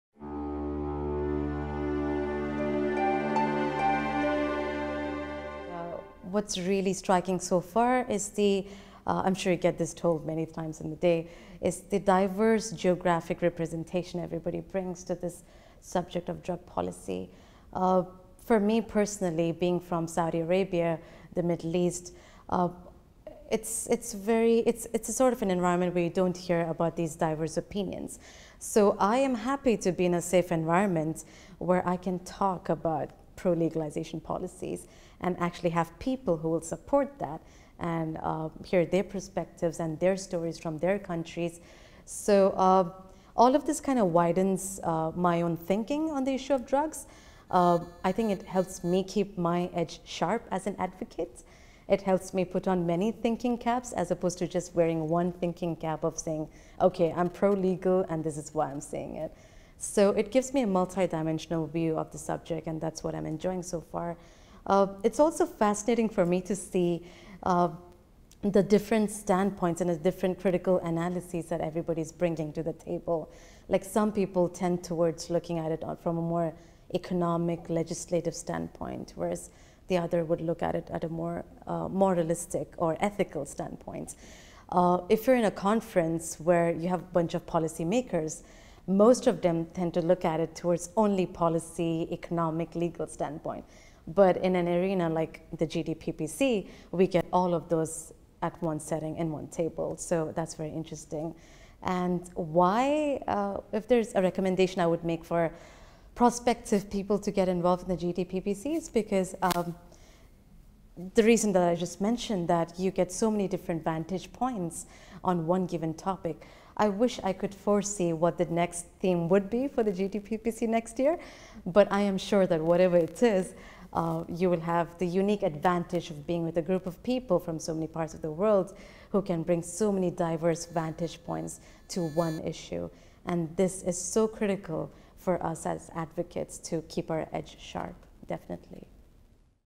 GDPPC Short Interviews: Nilofer Khan Habibullah
Short interview with Nilofer Khan Habibullah from Saudi Arabia, a semifinalist at Global Debate and Public Policy Challenge Budapest Forum 2014.

The Global Debate and Public Policy Challenge offers undergraduates across disciplines and continents an opportunity to explore issues of global importance from different points of view. Participants can become eligible to win a free trip to Budapest and a $10,000 scholarship.